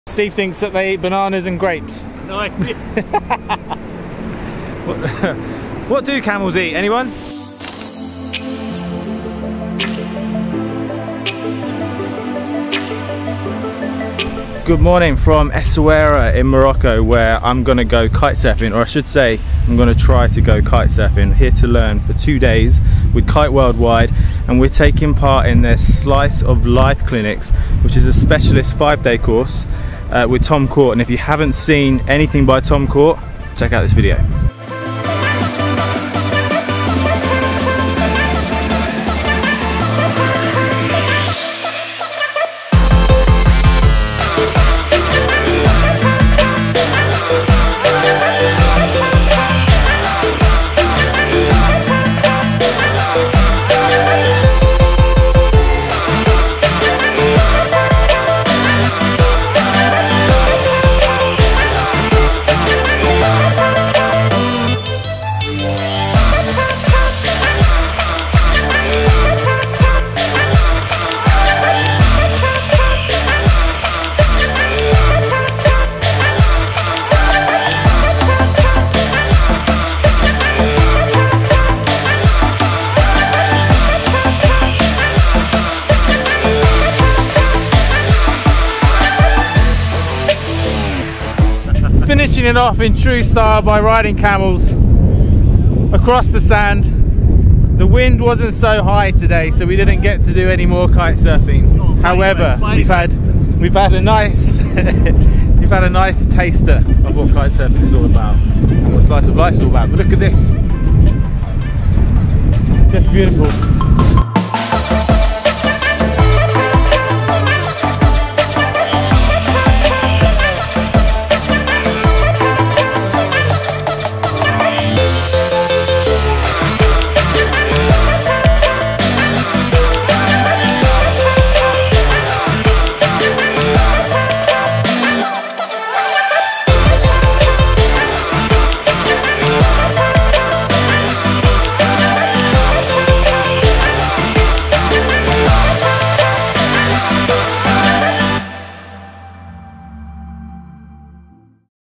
Learning to Kite-surf in Essaouira Morocco | with KiteWorldWide
The term “Slice of life” didn’t mean anything to me before this trip to Morocco. It’s the name of the kite-surfing camp run by KiteWorldWide, in collaboration with Tom Court. This was our two-day crash course in Kite-surfing, check it out!